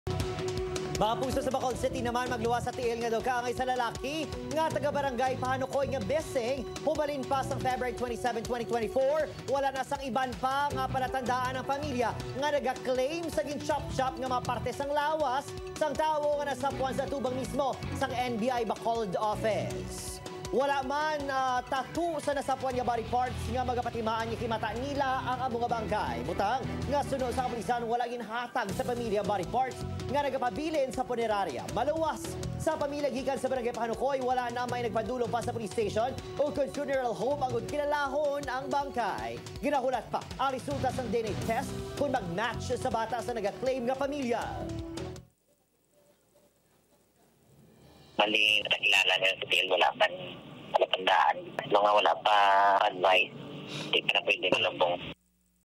One Western Visayas: Himata sang lalaki nga missing sa Brgy. Pahanocoy, nagbase sa tiil sang biktima
Himata sang lalaki nga missing sa Brgy. Pahanocoy, nagapati nga iya sang biktima ang mga utod nga parte sang lawas sang tawo pinaagi sa pagkapareho sang suno sa kapulisan.